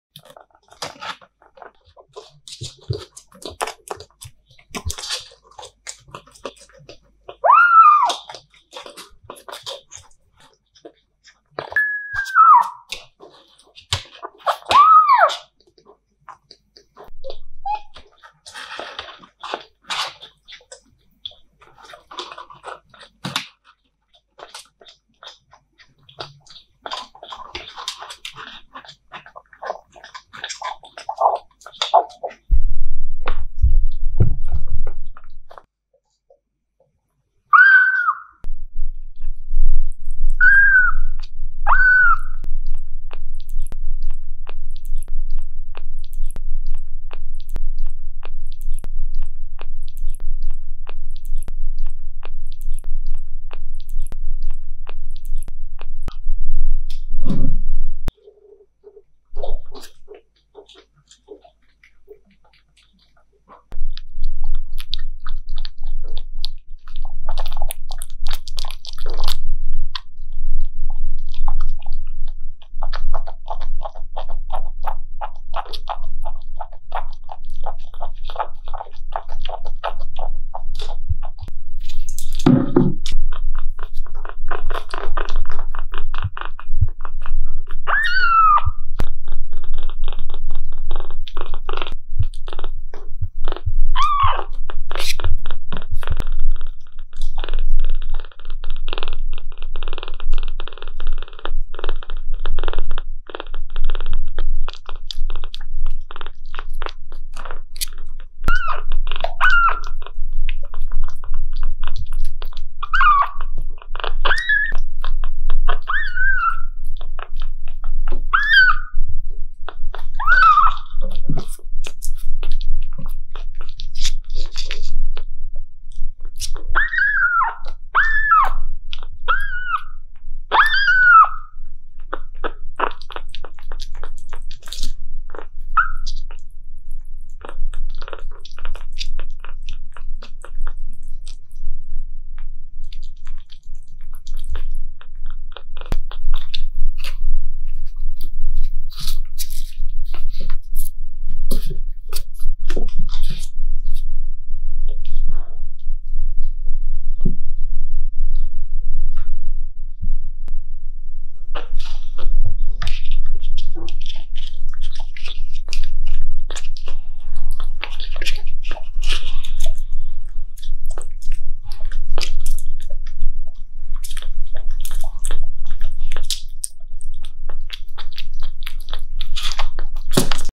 Cute Sugar Glider Rides Kitten
My cute baby sugar glider rides kitten with purring & meowing asmr video...

Thank you very much, wish you a happy evry day. (5000 LIKE for this videos)